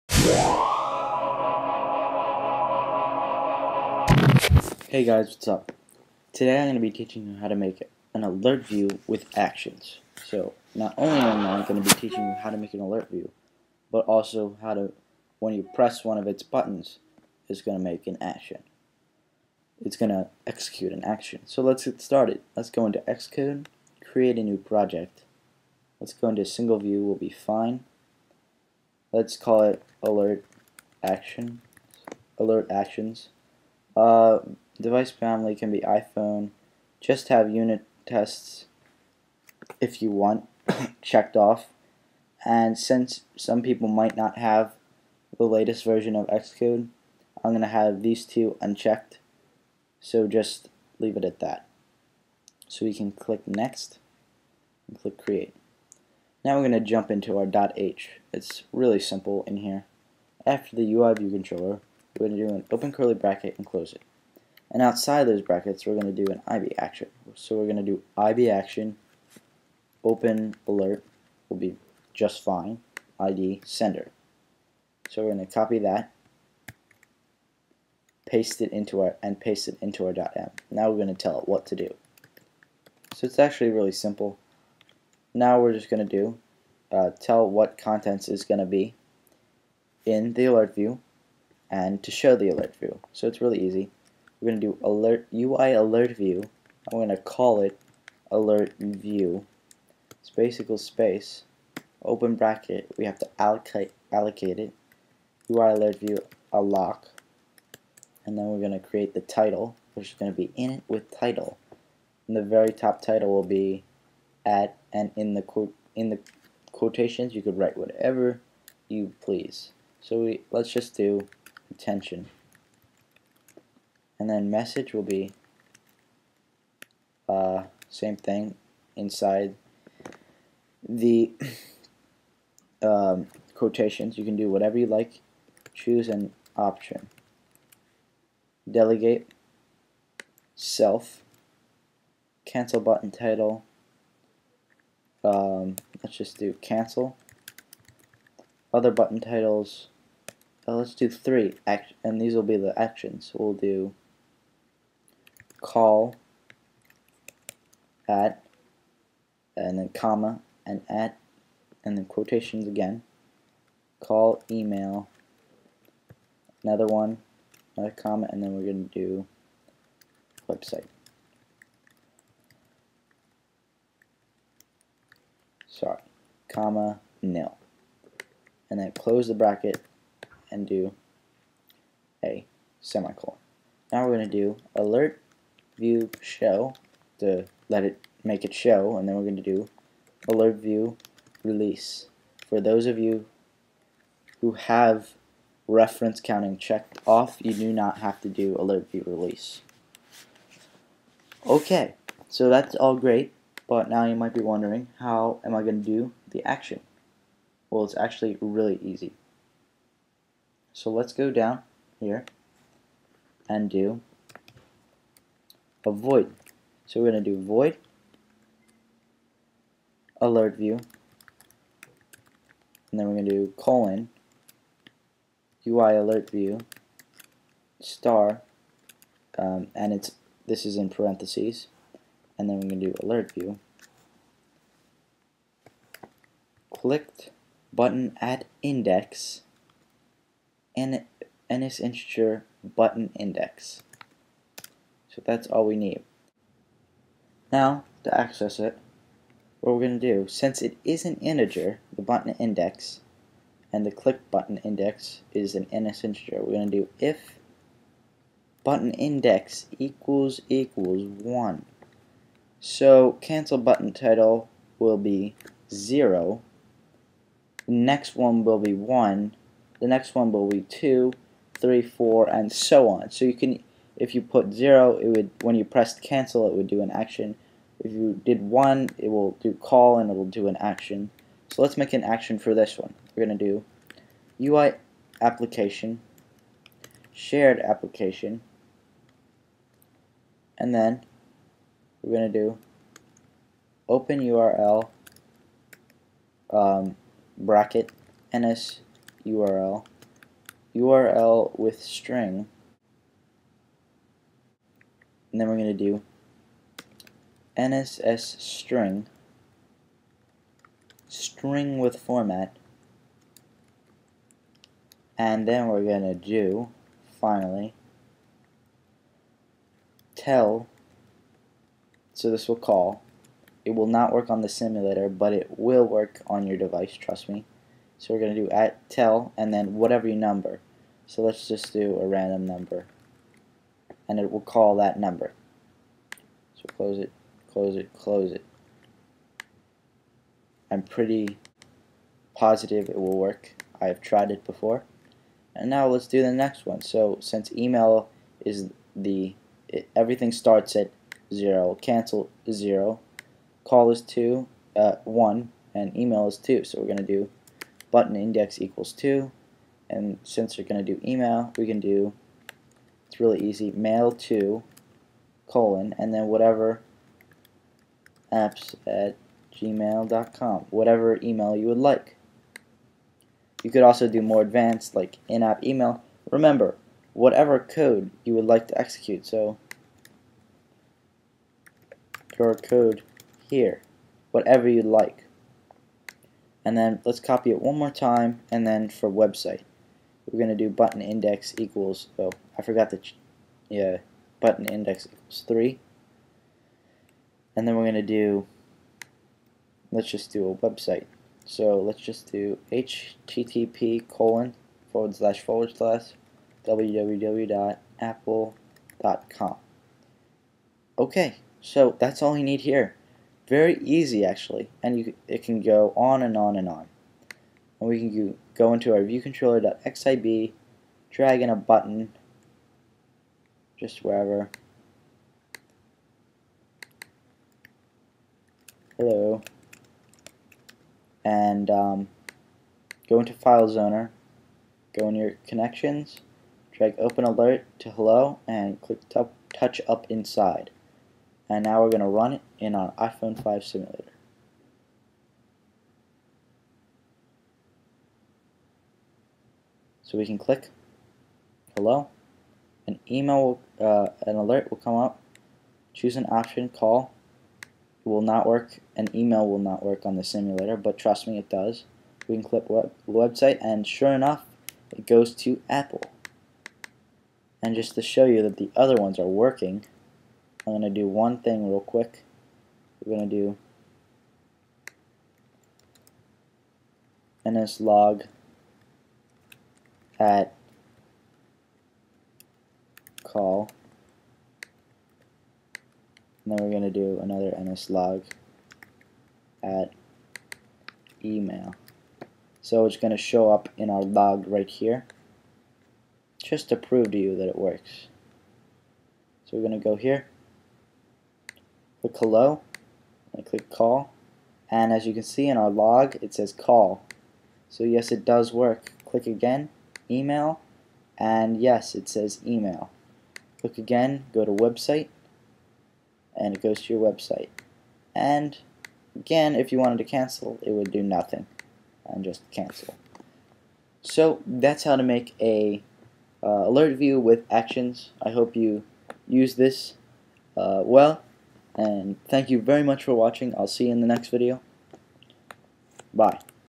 Xcode: How to Create an Alert View with Actions
How to create an alert view with buttons that execute actions with only a few lines of code.  Enjoy.